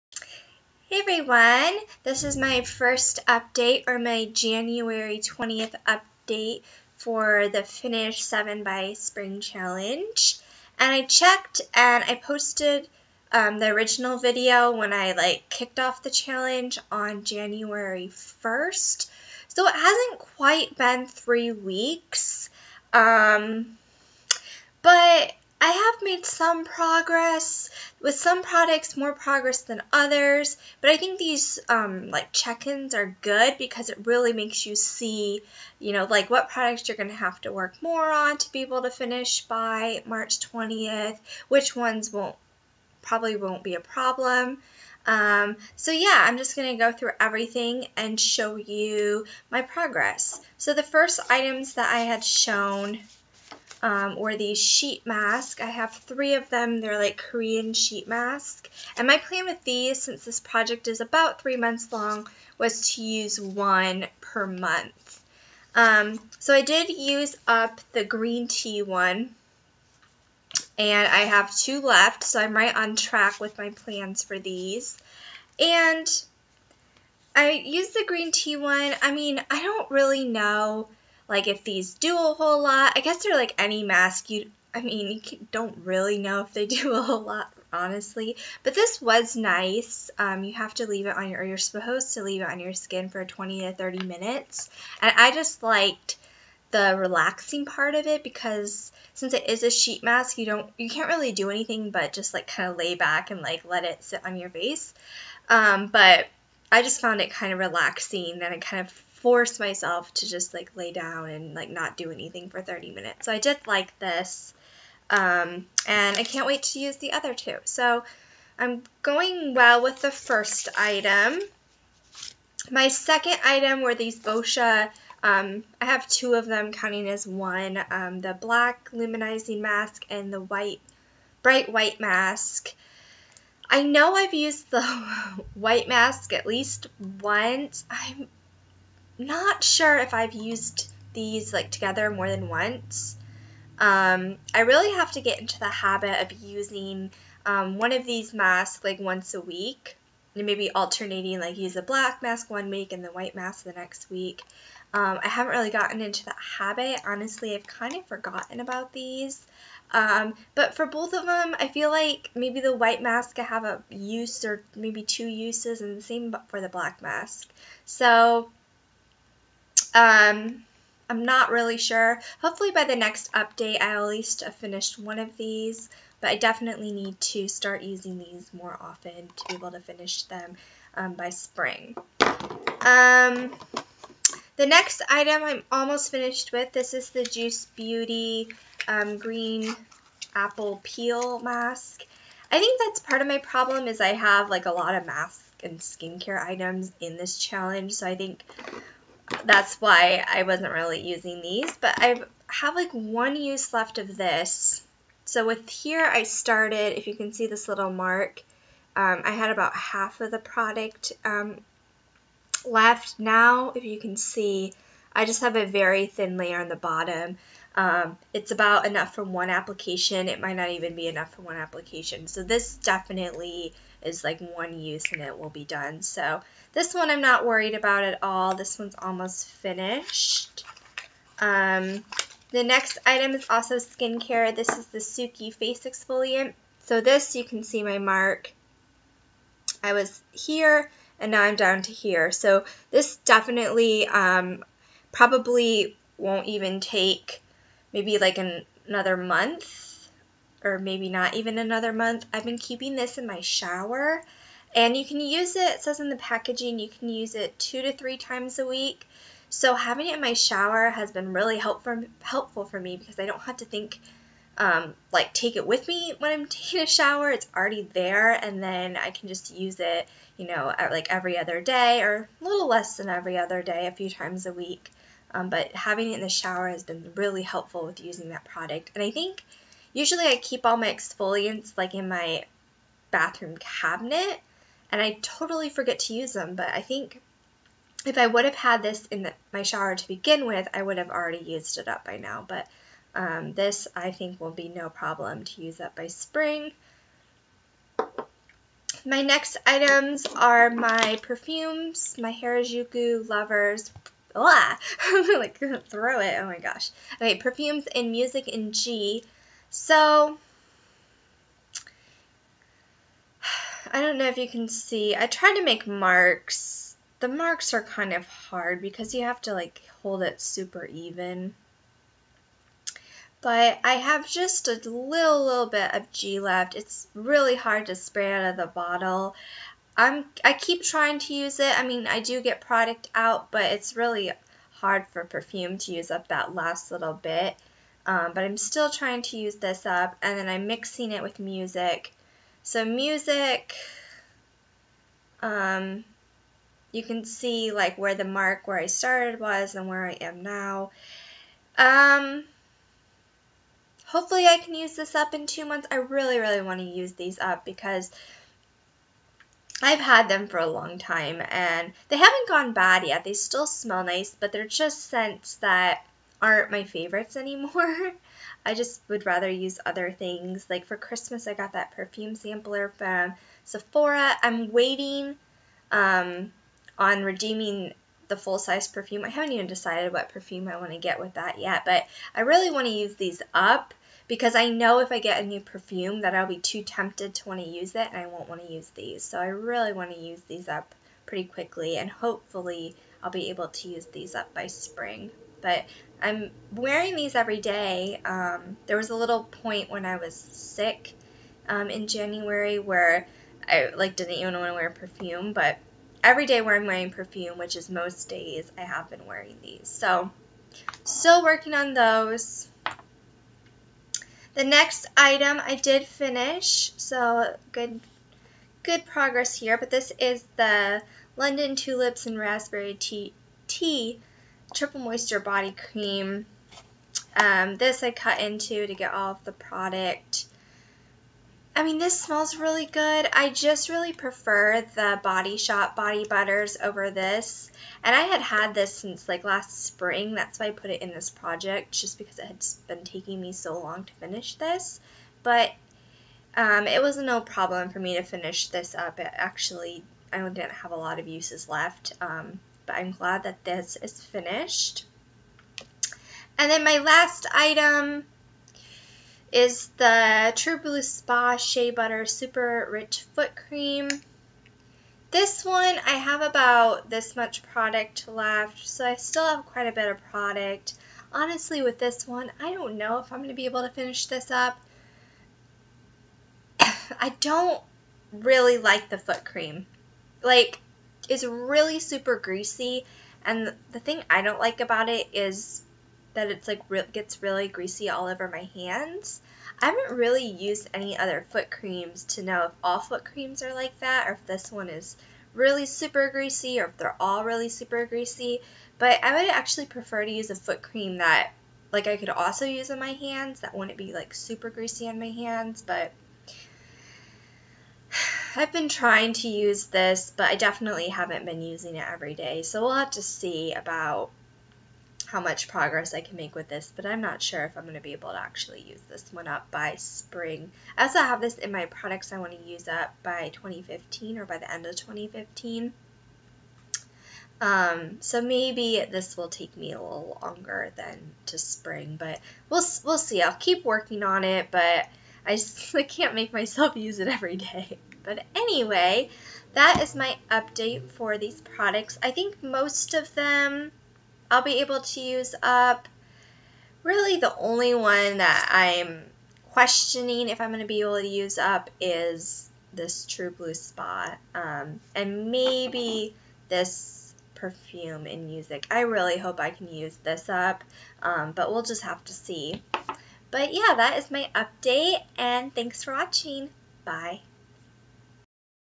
Finish 7 By Spring Update # 1
This is my first update for Finish 7 by Spring.

Products Mentioned:
3 Korean Sheet Masks
Boscia Luminizing Black Mask
Boscia White Mask
Juice Beauty Green Apple Peel Sensitive
Suki Face Exfoliate Foaming Cleanser
Harajuku Lovers G Perfume
Harajuku Lovers Music Perfume
Bath and Body Works London Tulips and Raspberry Rain Triple Moisture Body Cream
True Blue Spa Shea Butter Super Rich Foot Cream

All products were purchased by me or were gifts from friends or family.  All opinions are my own.